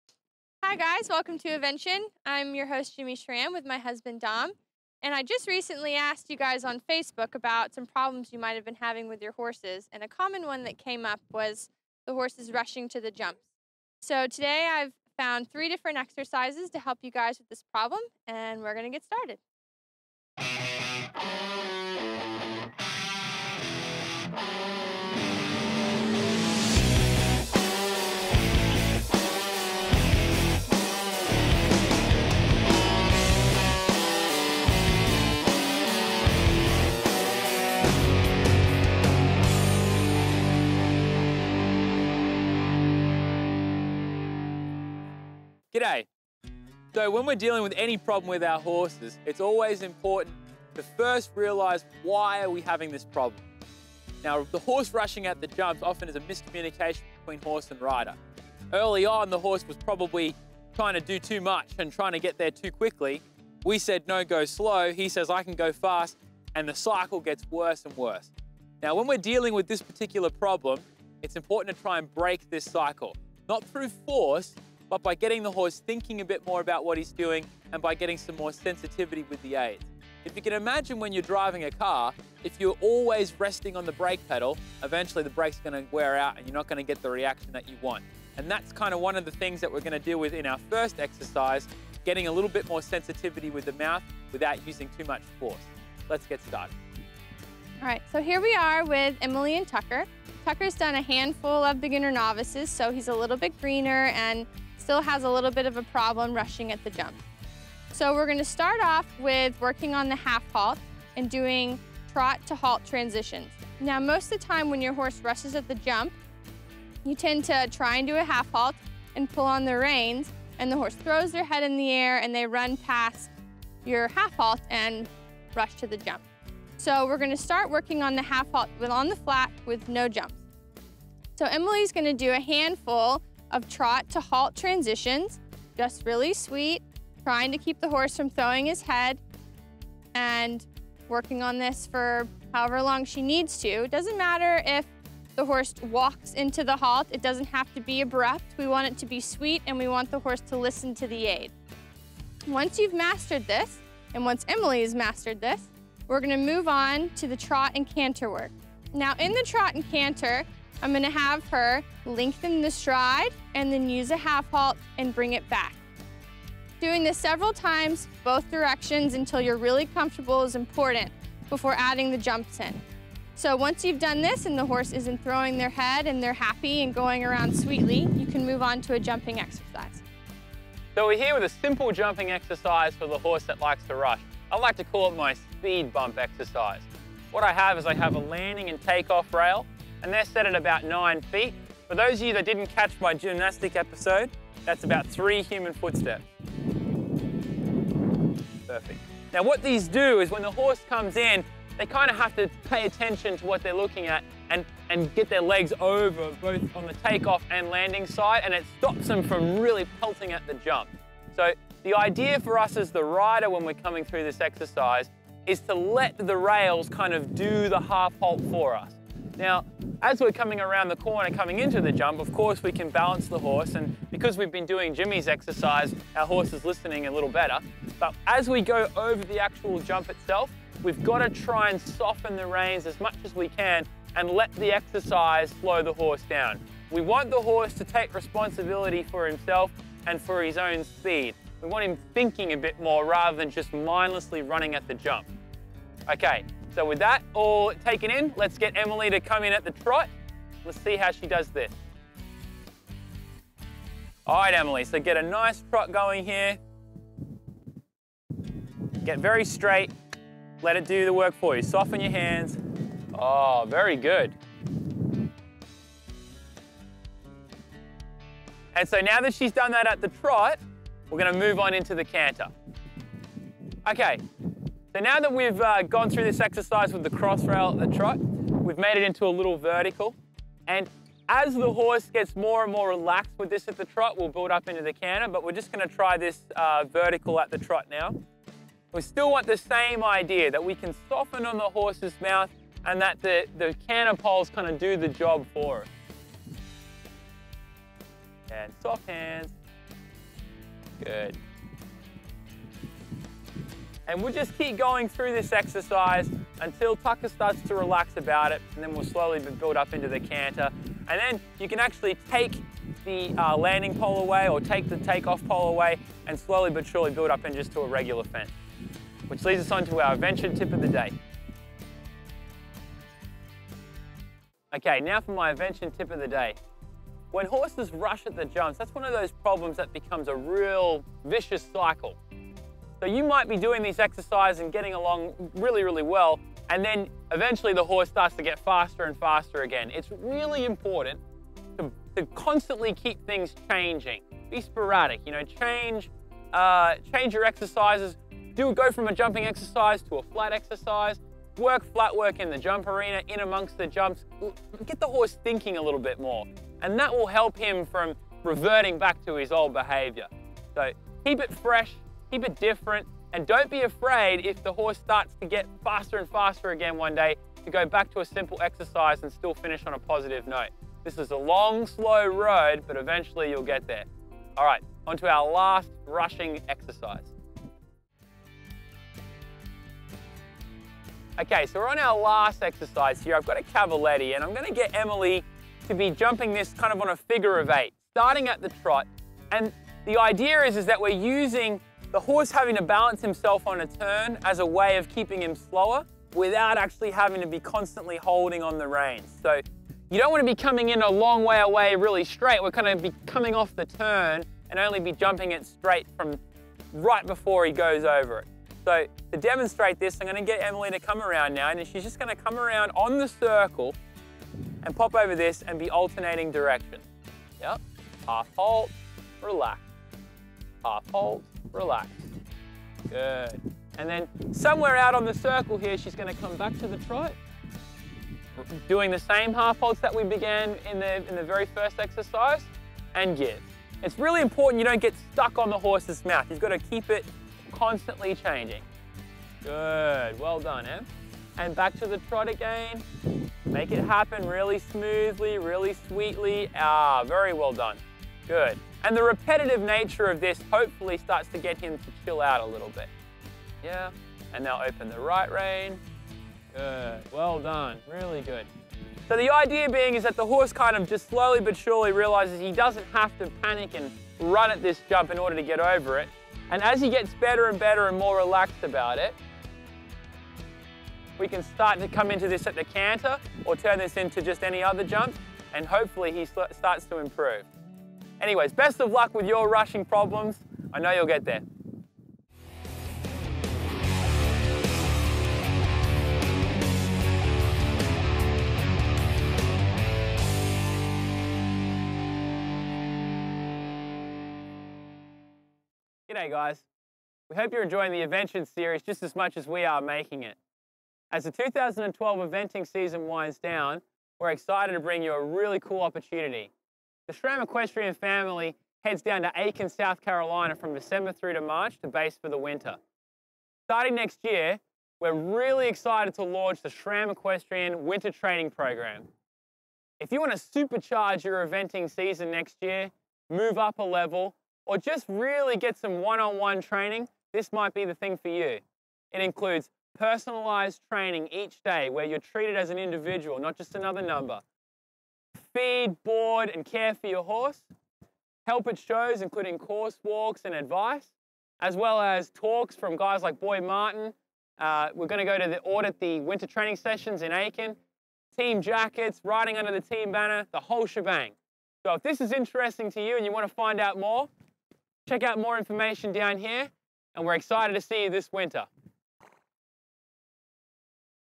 Rushing Fences: How to school a horse that rushes at jumps
Today on Evention we demonstrate how to school your horse over various jumps. This tutorial helps to teach inexperienced horses and riders how to slow your approach and take the jump with ease. We also have a new Evention Tip of the Day, and stayed tuned to the very end of this weeks's episode for an important message. Good Riding Mates.

In the comments below, gives us your thoughts and share with others this brand new series.

Evention is filmed and edited by Right Start Photography